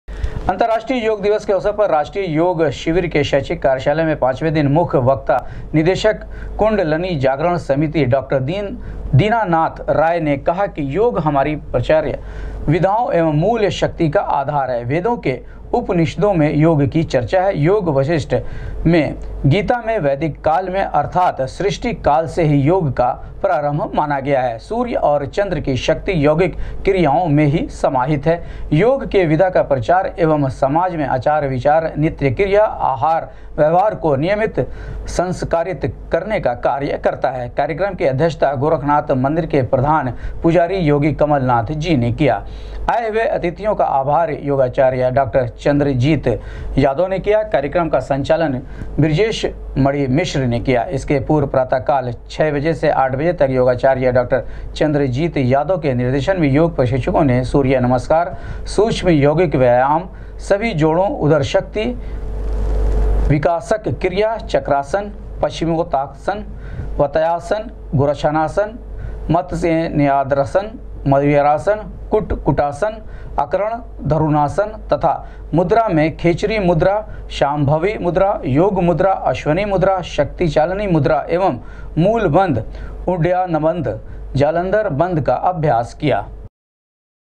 अन्तर्राष्ट्रीय योग दिवस के अवसर पर आयोजित साप्ताहिक योग शिविर में दी गयी योग के विविध कलाओं की
अन्तर्राष्ट्रीय योग दिवस के अवसर पर आयोजित साप्ताहिक योग शिविर में दी गयी योग के विविध कलाओं की जानकारी,मुख्य वक्ता डाॅ0 दीनानाथ राय ने कहा कि योग हमारी प्राच्य विधाओं एवं मूल शक्ति का है आधार।